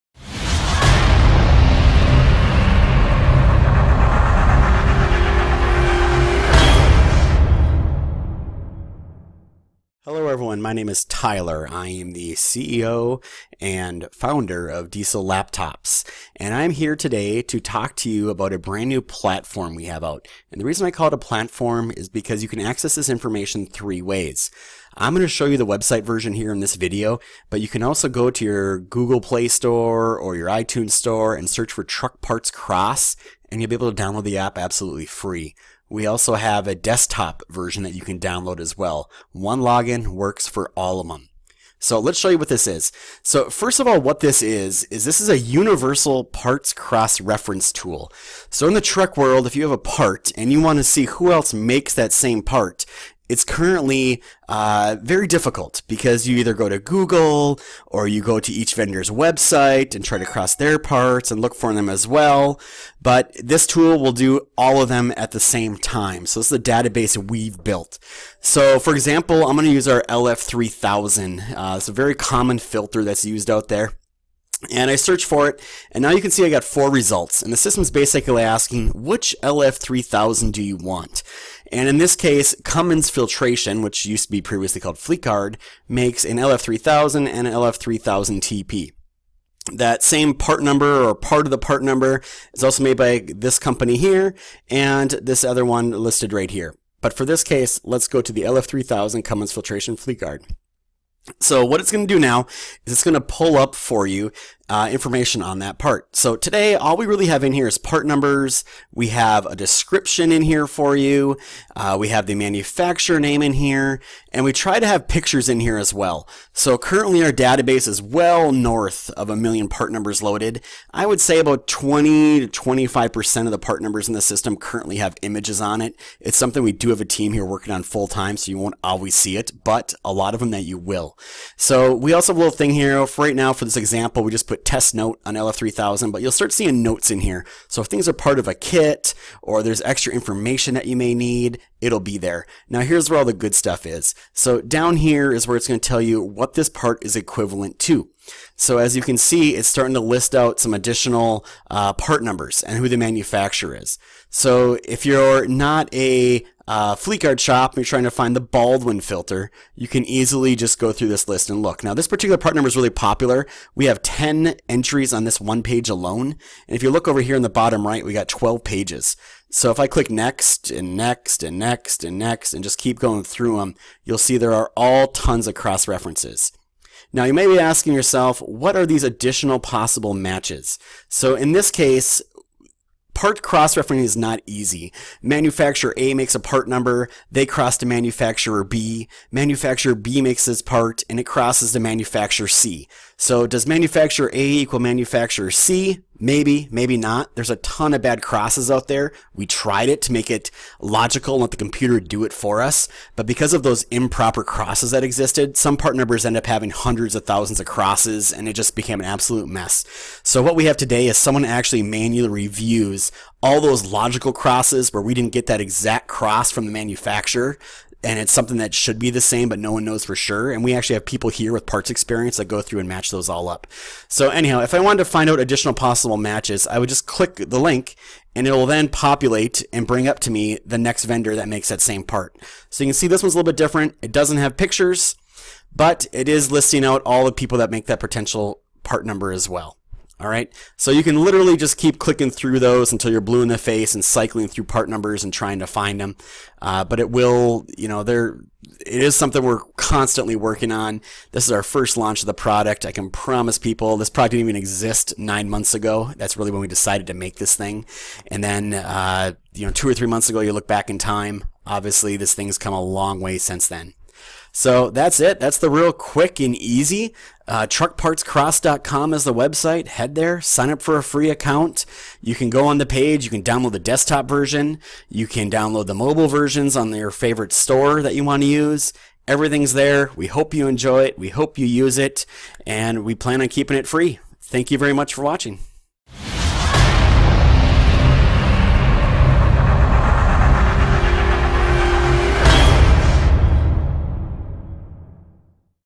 Truck Parts Cross - Web Demo
Take a look at how our Truck Parts Cross website works, the functionality it provides, how to use it properly.